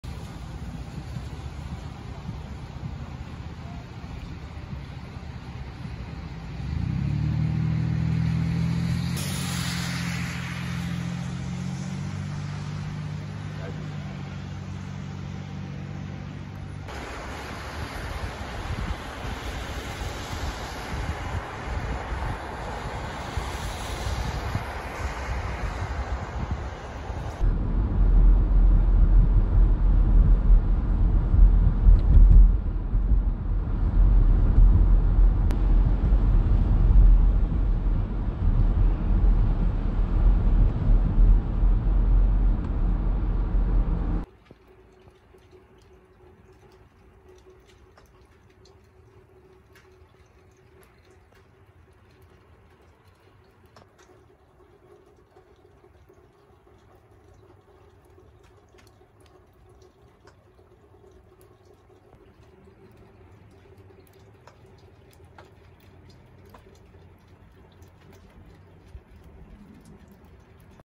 3-14-2020 West Lafayette, IN Heavy Snowfall
Heavy snow impacted much of Central Indiana